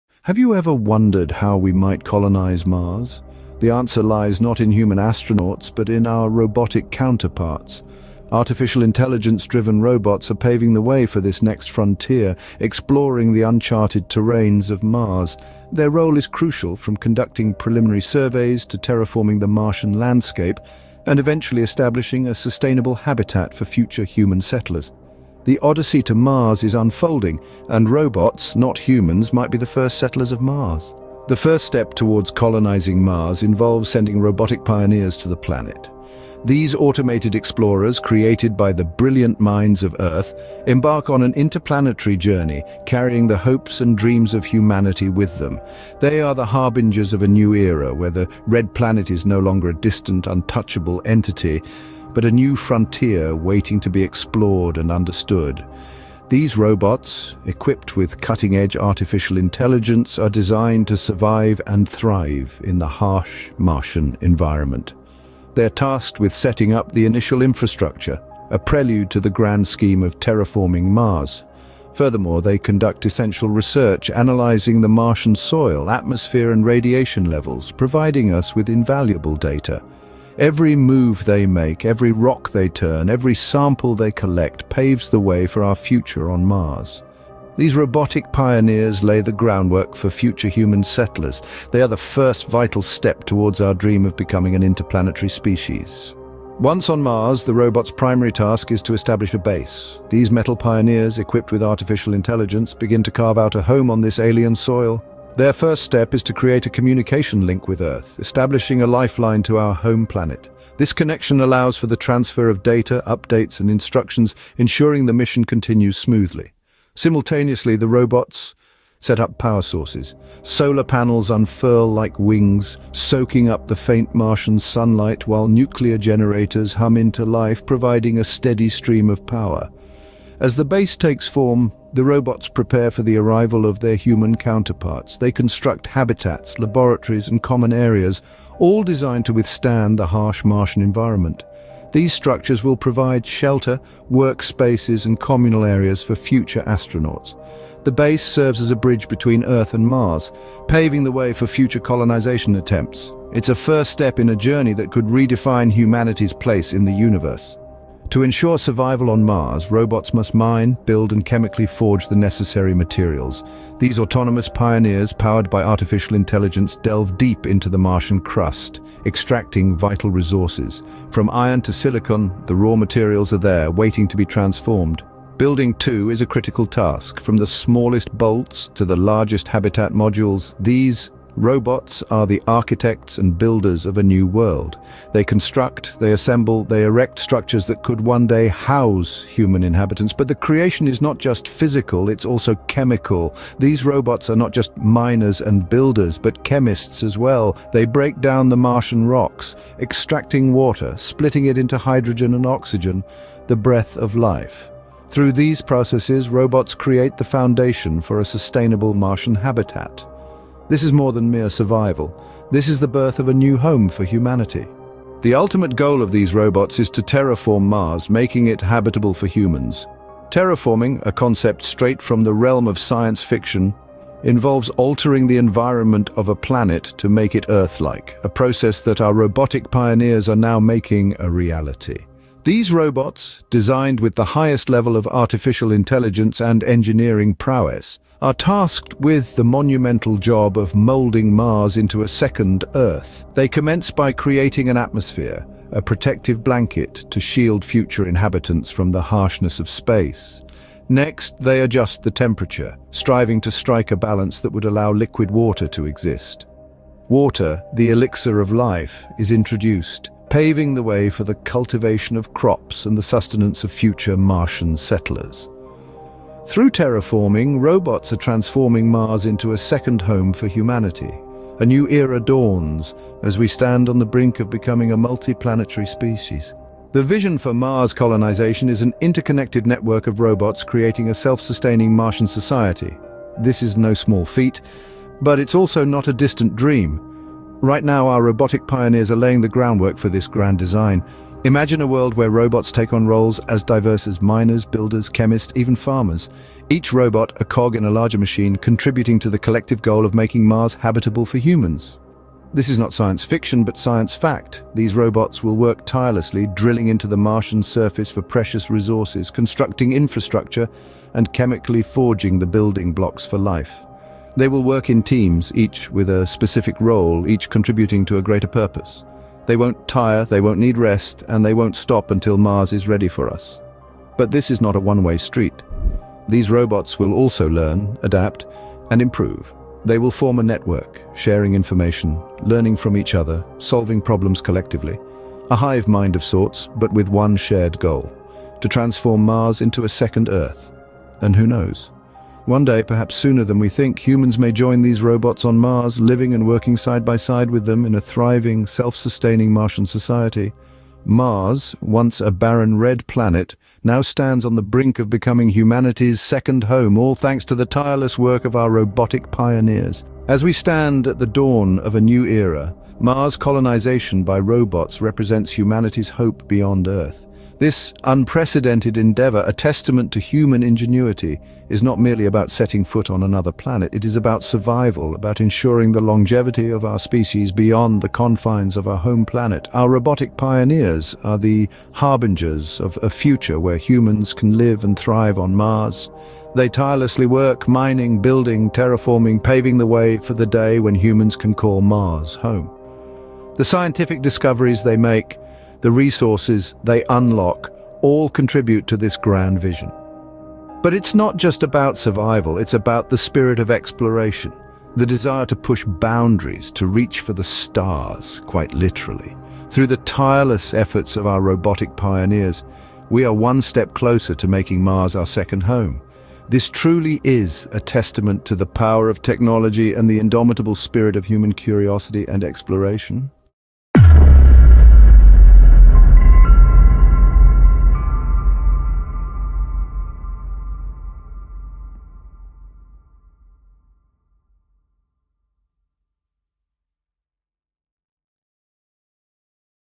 AI-Powered Robots Are Leading to Mars, are they? (Sci-Fi Documentary)
In this captivating documentary, "AI-Powered Robots Leading the Colonization," we embark on an incredible journey to the Red Planet, Mars. Join us as we delve deep into the world of cutting-edge Artificial Intelligence and the remarkable robots that are at the forefront of making Mars our second home.

Explore the breathtaking Martian landscape and witness the extraordinary capabilities of AI-enabled robots as they build, terraform, and conduct groundbreaking scientific experiments. From establishing bases to harvesting vital resources like water and oxygen, these robots are the pioneers of a new era in space exploration.

Discover the secrets behind their unprecedented achievements, and witness the breathtaking visuals that showcase the future of interplanetary colonization. Will Mars become humanity's next home? Find out as we uncover the astounding potential of AI-powered robots in this epic journey.

Don't miss out on this extraordinary adventure into the unknown! 🌌🤖🌠